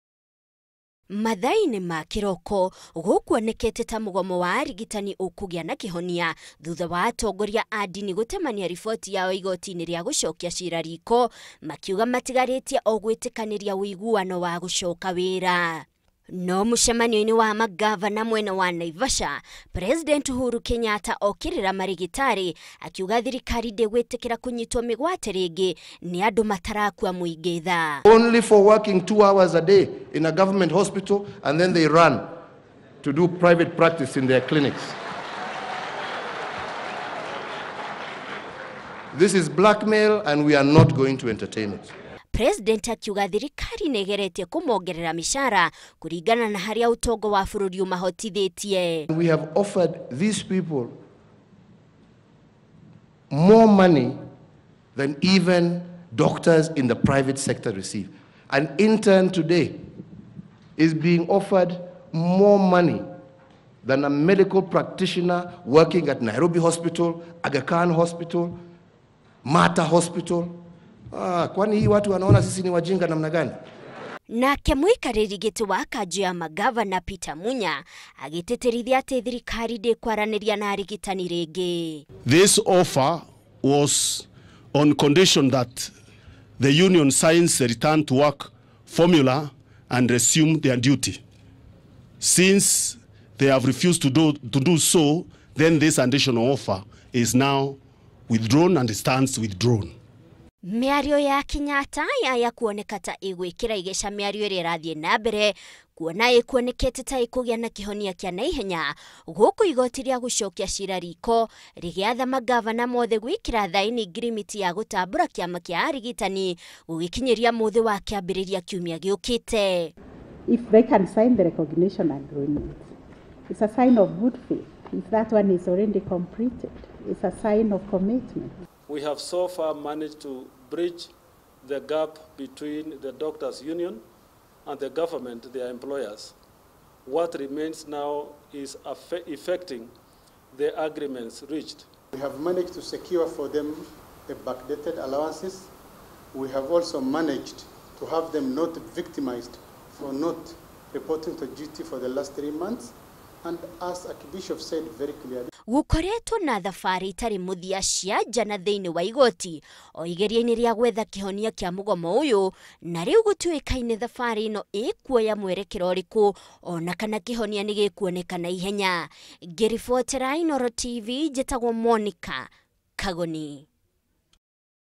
Thirikari kweheria riendekithia ria kuongerera arigitari mucaara gicunji kia 40%
Miario ya kunina mugomo wa marigitari ni iguthite  ihiga. Thirikari oro ho ni yeheria riendekithia riria irahete marigitari ria kumongerera mucaara na gicunji kia mirongo ina hari igana.
Riu thirikari yugiite ikurumanirira na marigitari umwe kwa umwe na aria metikira gucooka wira na ihenya ota uria kwahoteka, noo mekuheo nyongerera iyo.